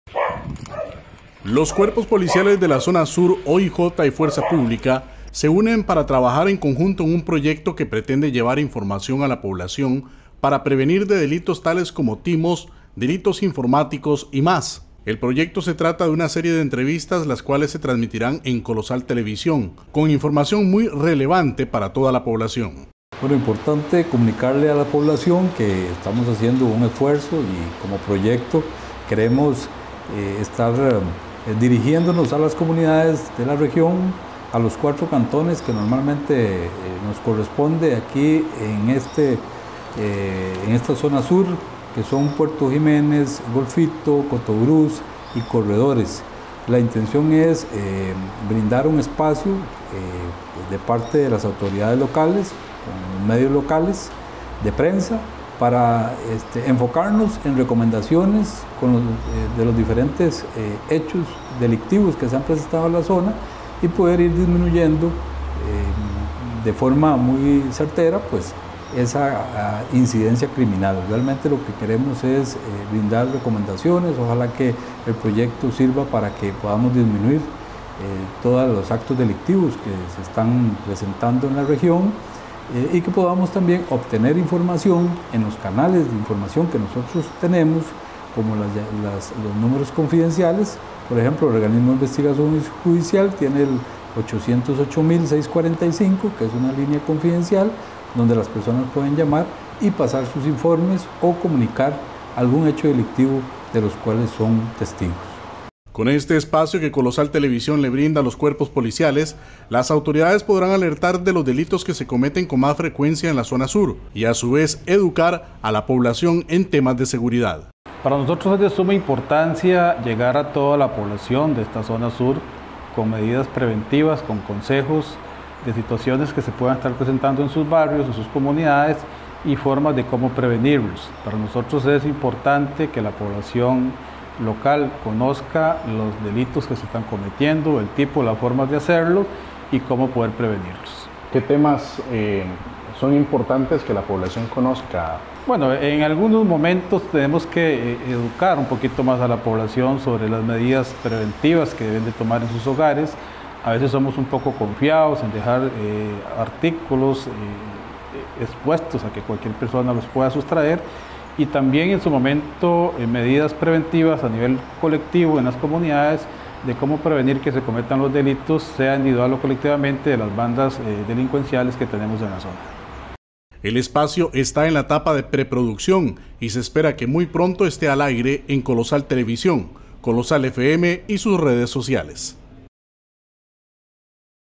CUERPOS POLICIALES TENDRÁN ESPACIO EN COLOSAL
Con el fin de informar y alertar a la población sobre la criminalidad y su desarrollo en la zona sur, los Cuerpos Policiales han solicitado al Grupo Colosal, ayuda para difundir información. En Colosal les hemos abierto la posibilidad y muy pronto tendrán su programa.